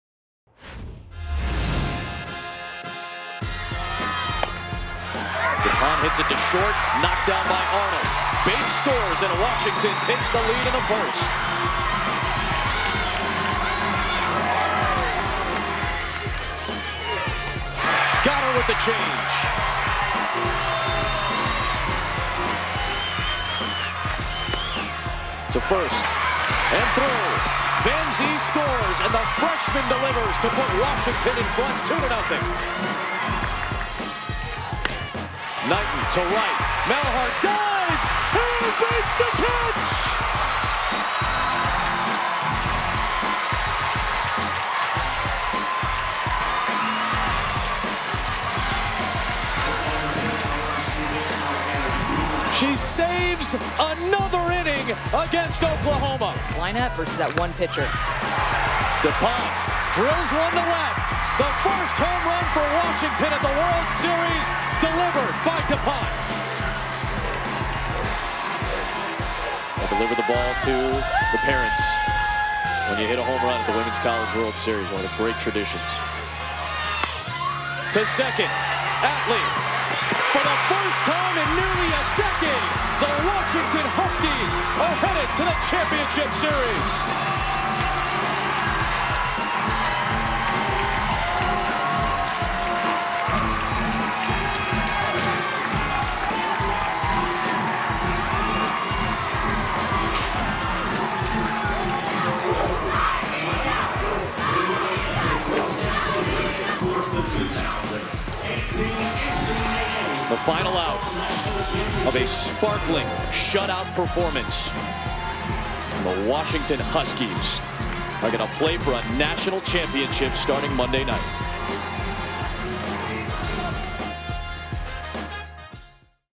Highlights: Washington blanks Oklahoma again, punches ticket to WCWC championship series
UW starter Taran Alvelo spins a shutout, Julia DePonte knocks in two RBIs and Washington defeats two-time defending champion Oklahoma 3-0 on Sunday to advance to the WCWS championship series beginning Monday in Oklahoma City.